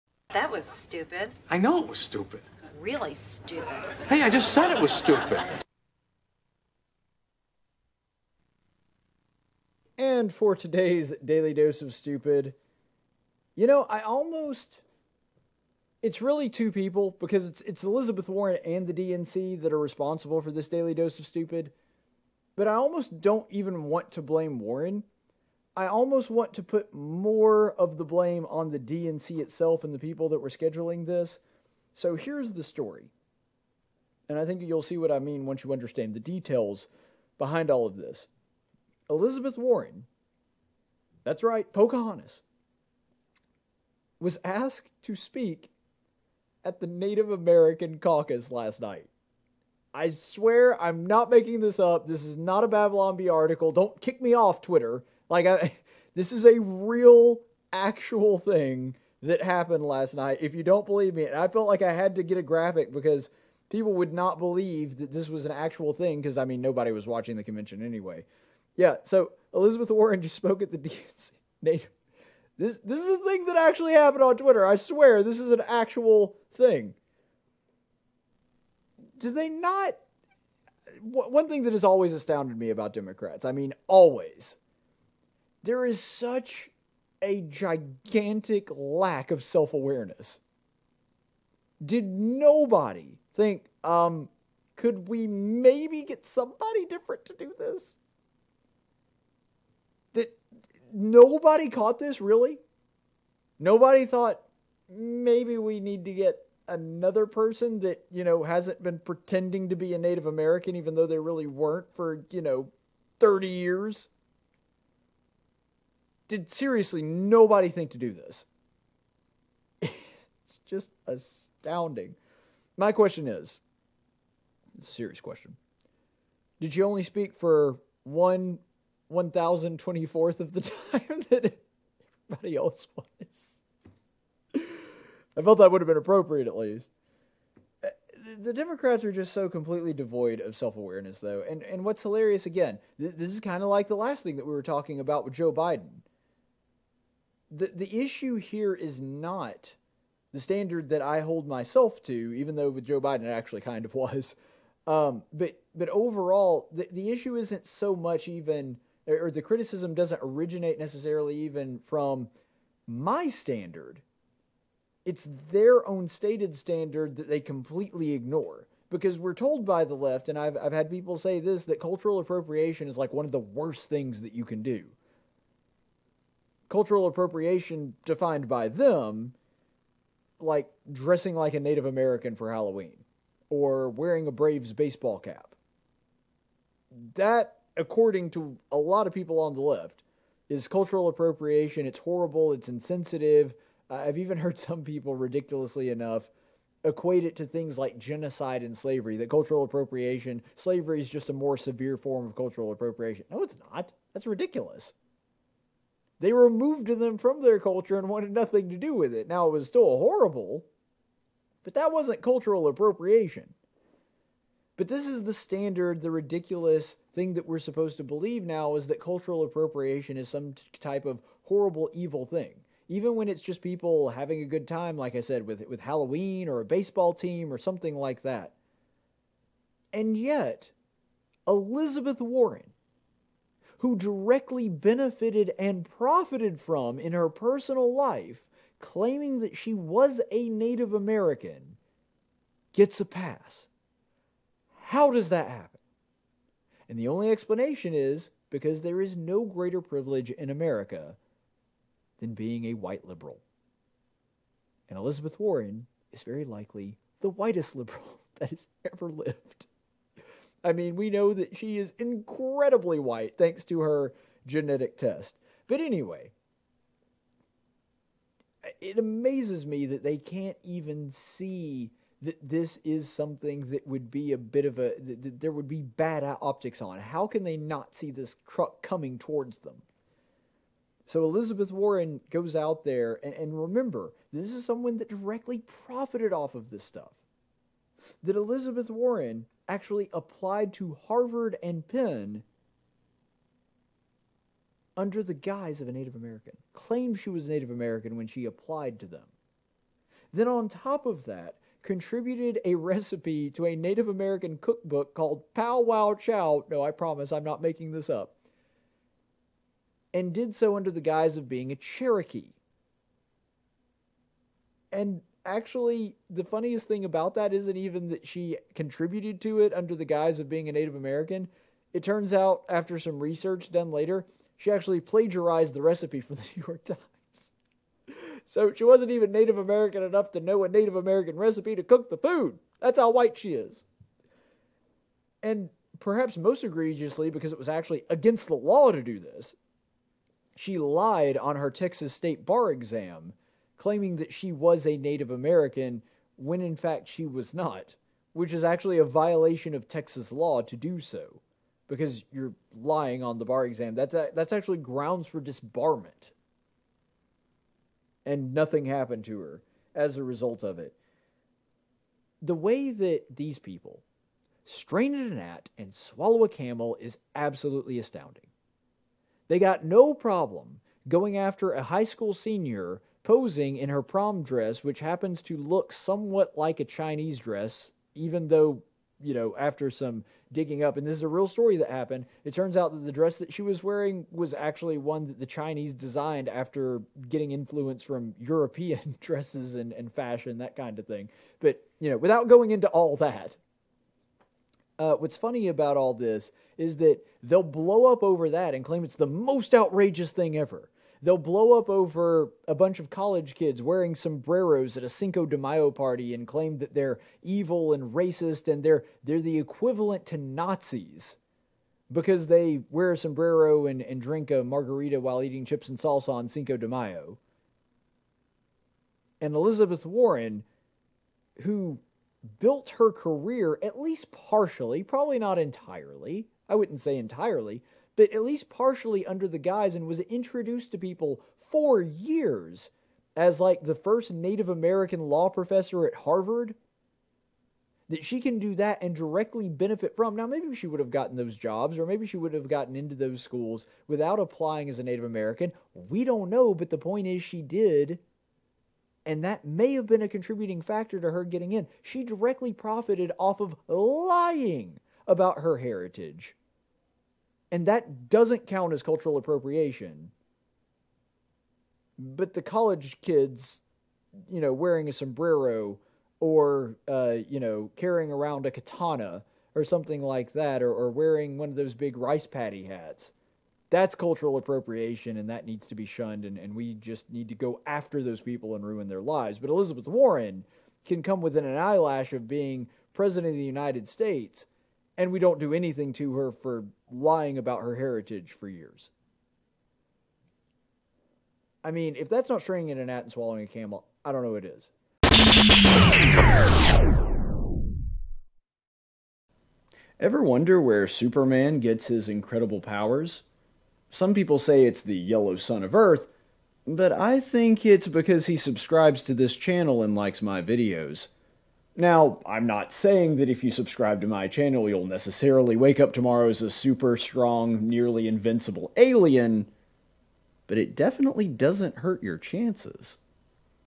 DDoS- Elizabeth 'Fauxcahontas' Warren Speaks to the Native American Caucus
The DNC gets Sen. Elizabeth Warren to speak to the Native American Caucus, despite the fact she lied about being a Native American for decades.